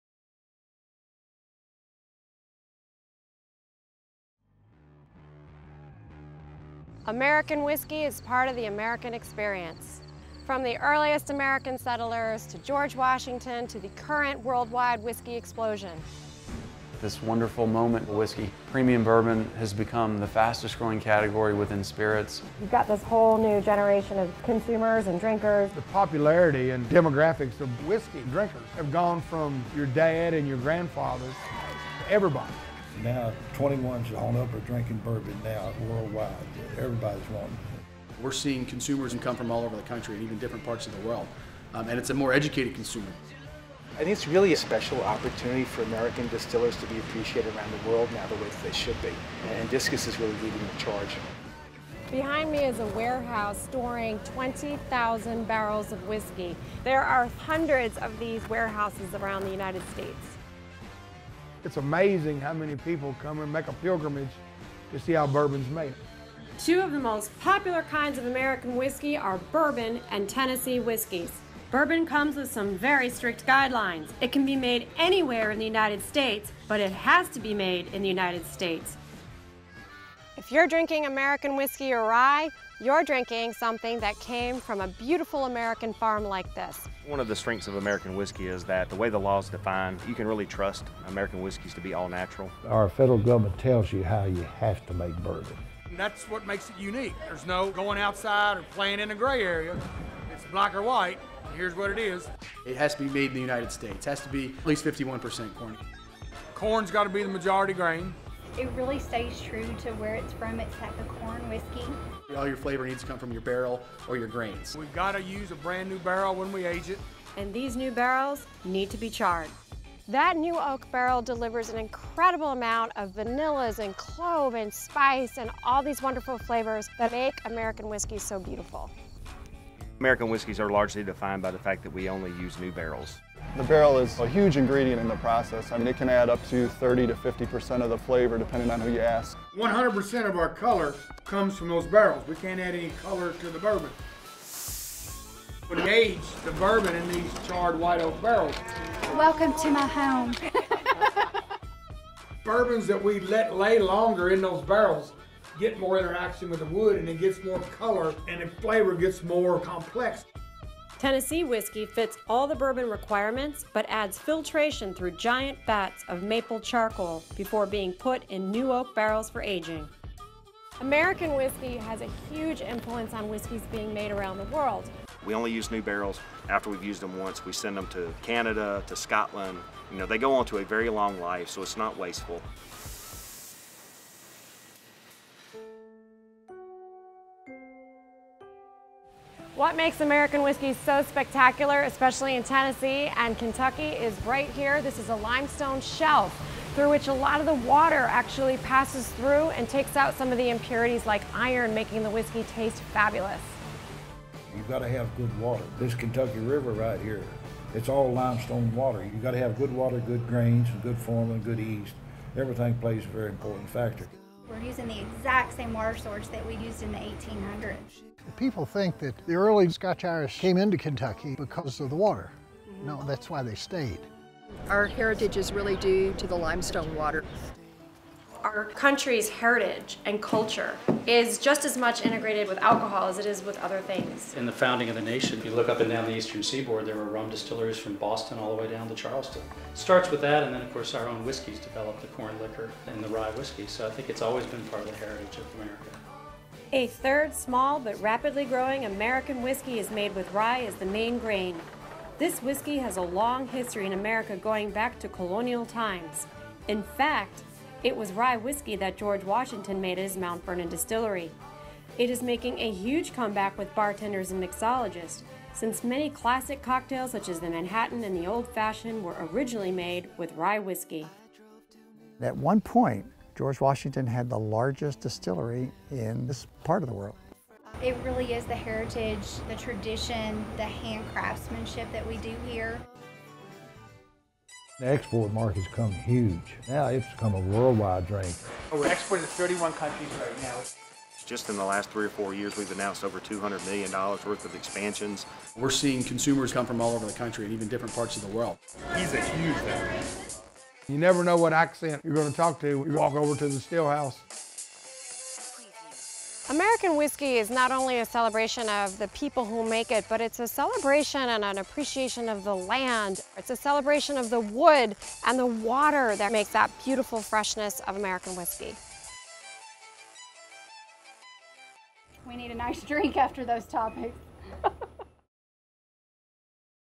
American Whiskey Trail
From the colonial era, where Whiskey had an important economic and social function in the fabric of the community, to the Whiskey Rebellion, through Prohibition and into modern times, spirits have played a sometimes controversial but always fascinating role in our nation’s history.